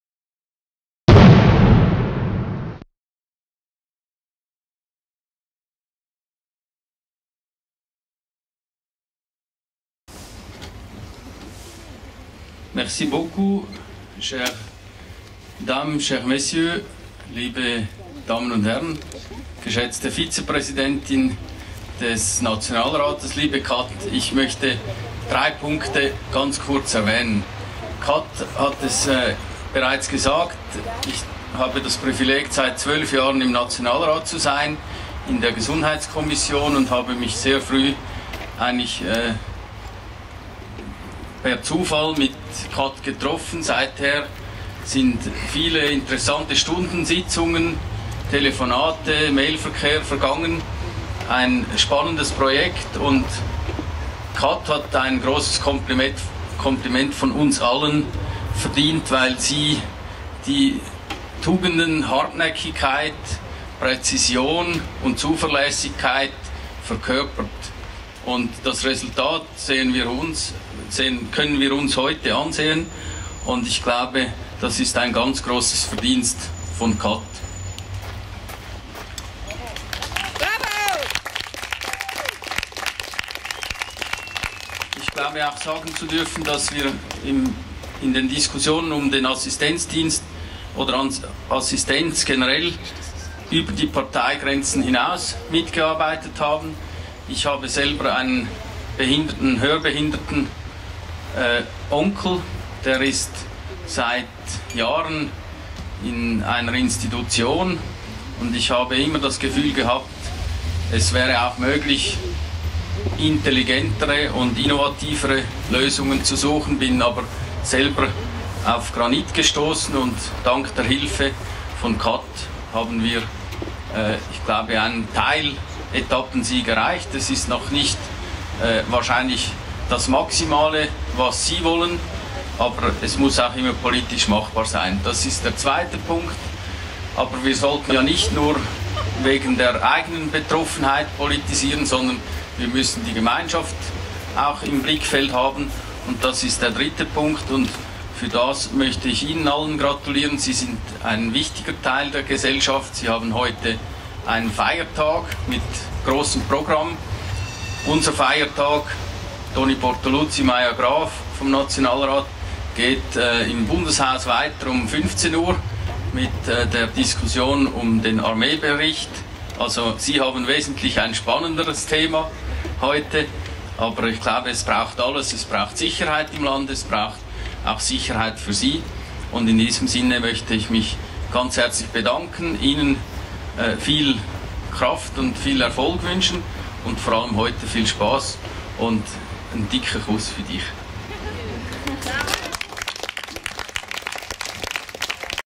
Rede Jürg Stahl am Festival Handicap im Kocherpark (Deutsch mit Untertiteln für Gehörlose)
Rede von SVP-Nationalrat Jürg Stahl am Festival Handicap im Kocherpark zur Einführung der Persönlichen Assistenz in der Schweiz.